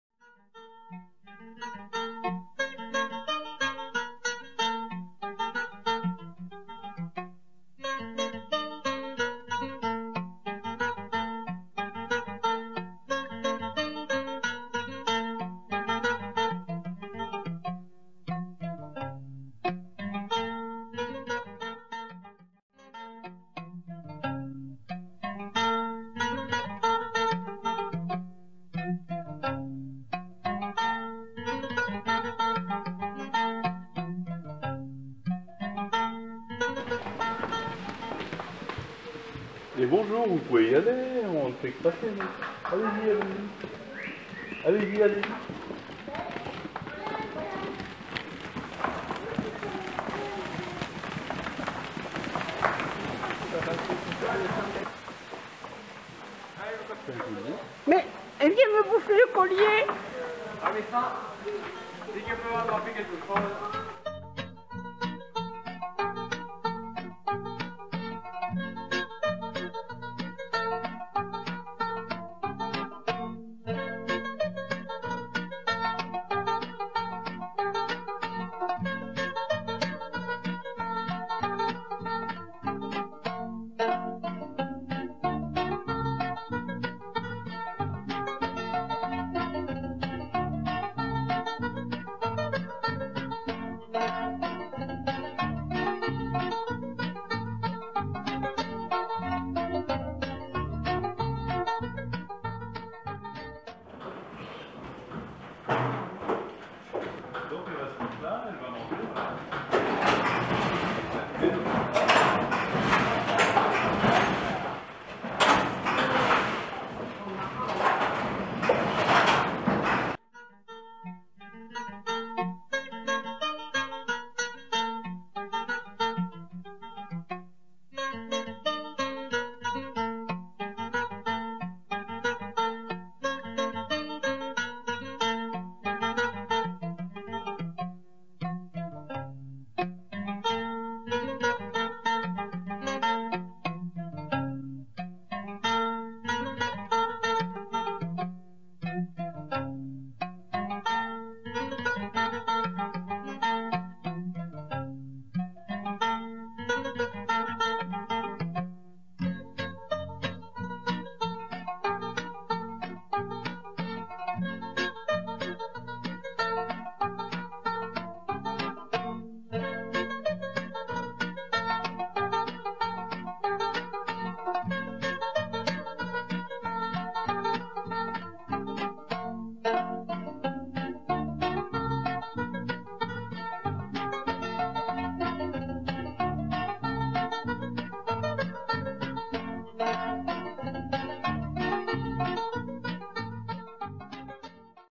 2016 06 03 les chèvres le film durée 3 14 à SAINT BEAUZEIL Tarn et Garonne 82
Sortie du 3 juin 2016 la chèvrerie à SAINT BEAUZEIL
Les photos et plus de renseignements sont disponibles sur le site de la DREB